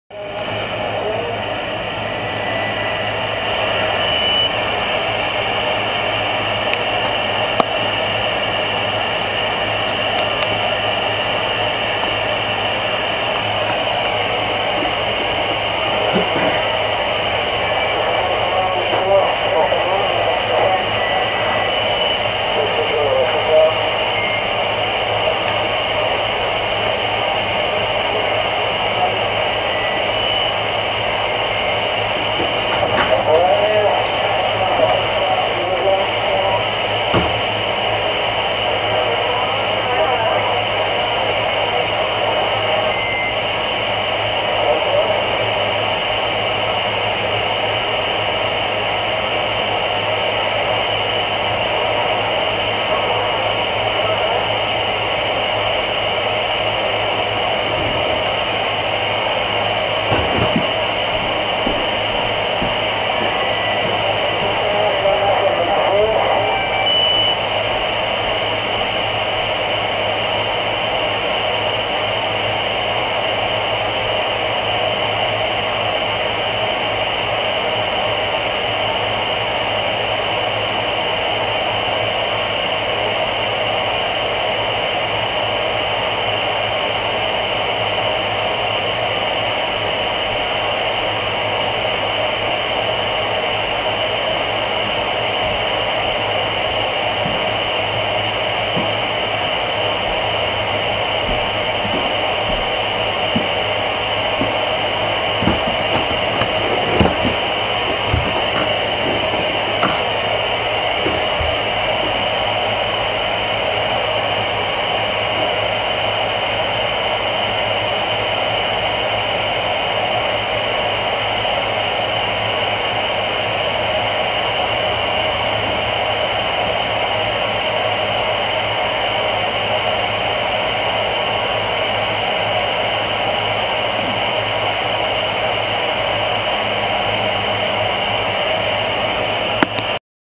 25.655 MHz AM Taxi Radio Dispatch 25MHz Band Freeband CB Land Mobile Radio - Received 9 Aug 2012
Taxicab Radio Dispatch on the 11-meter freeband below CB radio channel 1 using modified 10-meter ham radio gear - "band A" on most export radios.  Received on a CRE8900.